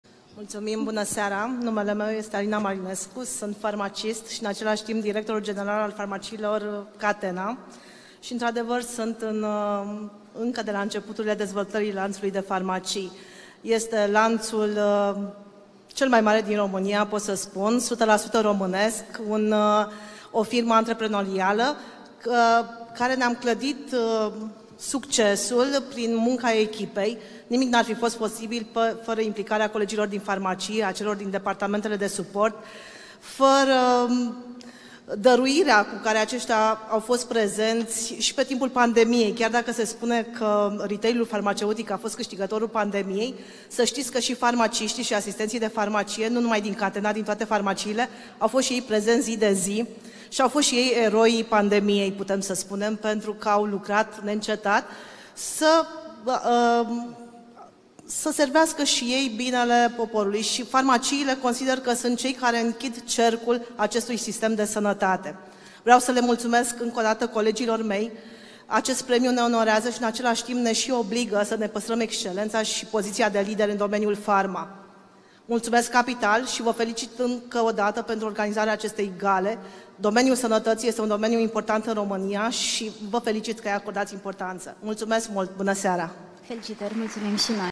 Alina Marinescu, Catena, la Gala Performeril din Sănătate
Este cel mai mare lanț de farmacii din România, numărând în prezent 900
de farmacii, peste 6.000 de angajați și mai bine de cinci milioane de clienți la
nivel național. Cu toate că piața în care Catena activează este foarte
concurențială, managerul companiei, Alina Marinescu este de părere că
aceasta este un lucru pozitiv, deoarece pozitia de lider în domeniu retailului
farmaceutic obligă la standarde ridicate și eficientă maximă. În numele companiei, directorul general Alina Marinescu primește din partea Revistei Capital Premiul pentru dezvoltarea celui mai mare lanț de farmacii din România